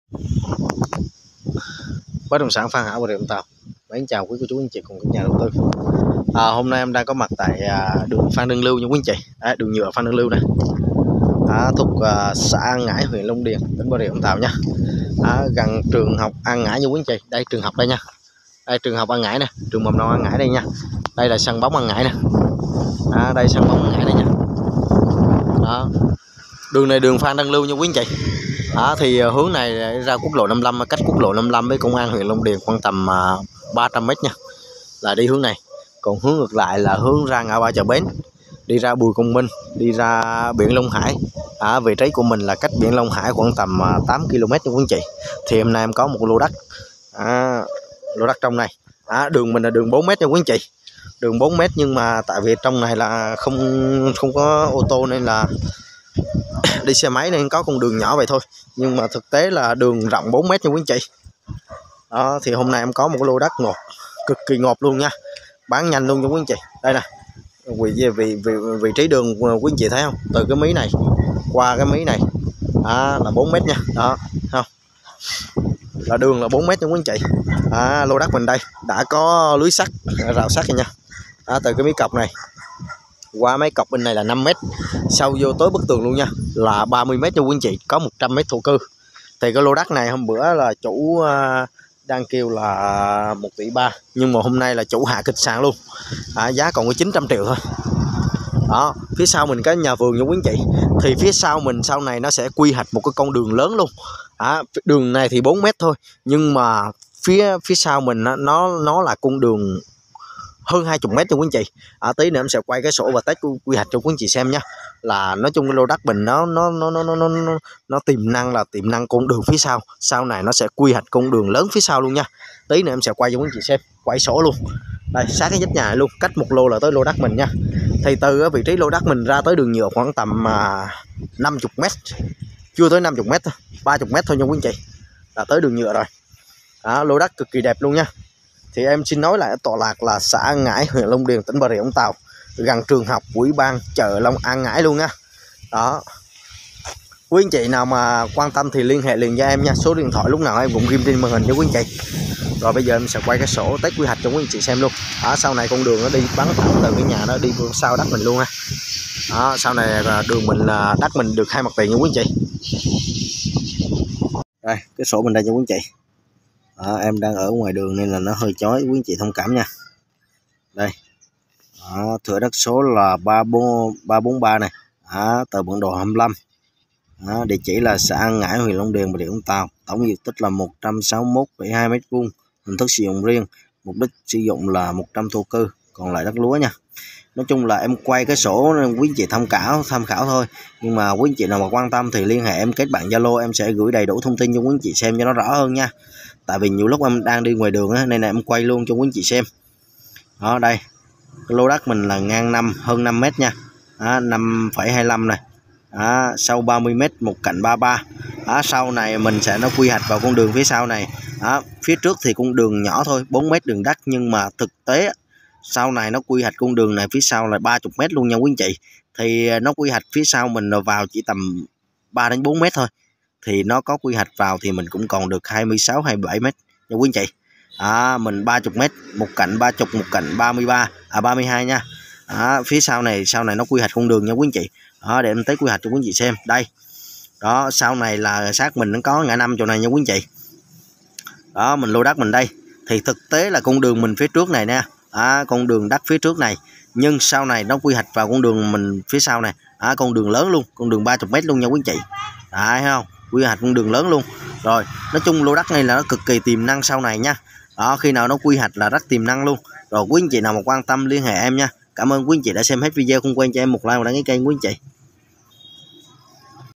lô đất đầy tiềm năng. sát Trường học chợ ủy ban . 5x32.100tc giá chốt 900tr. LH liền nhé
đất thuộc xã an ngãi huyện Long điền BRVT gần tất cả các tiện ích như trường học chợ ủy ban vvv. quy hoạch ra 2 mặt tiền trước sau là hốt bạc nhé. mua để dành cho con là quá ok lun ạ tổng DT ngang 5m dài 32m có sẵn 100m thổ cư giá 900tr chốt nhanh ạ.lh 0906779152 dắt xem đất gặp trực tiếp chủ làm việc nhé